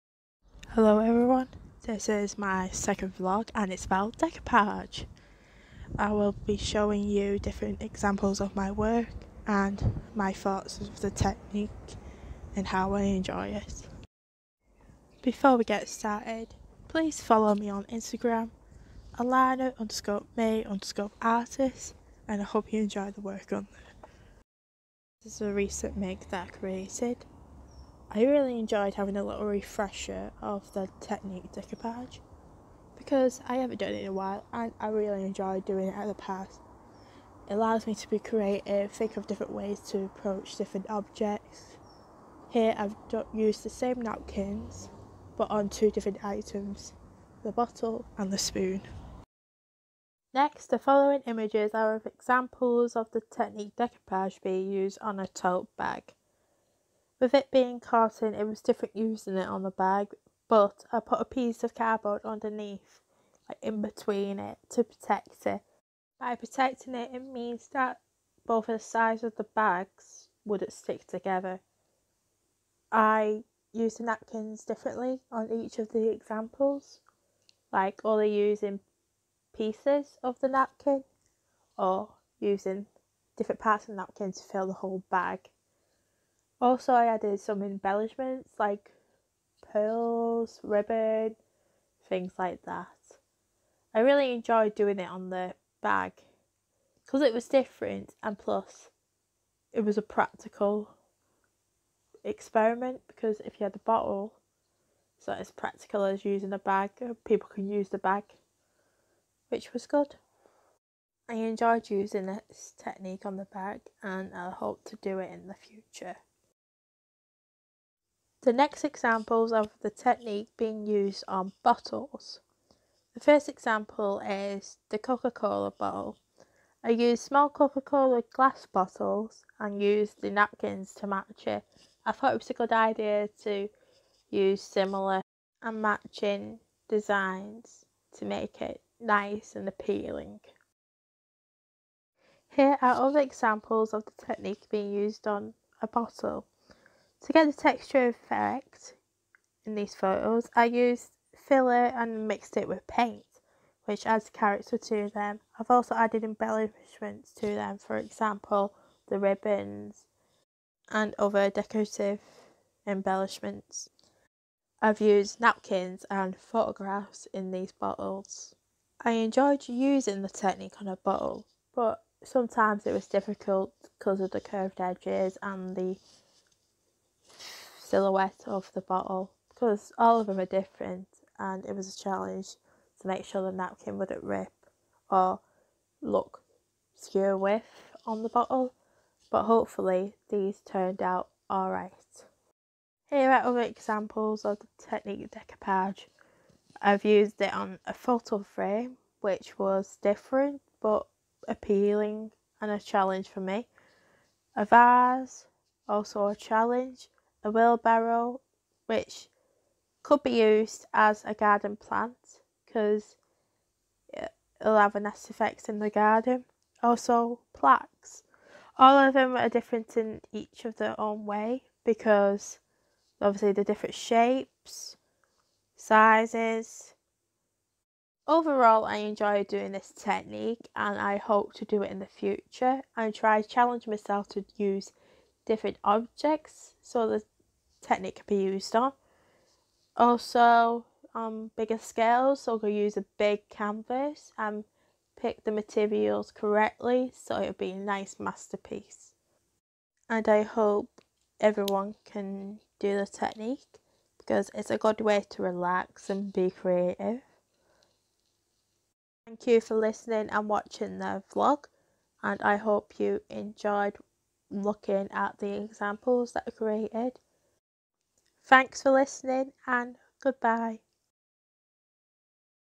VLOG - Decoupage (24th June 2019)
Hi everyone,
Here's another vlog about the work I did with the technique decoupage.

Work made by Alannah Brown.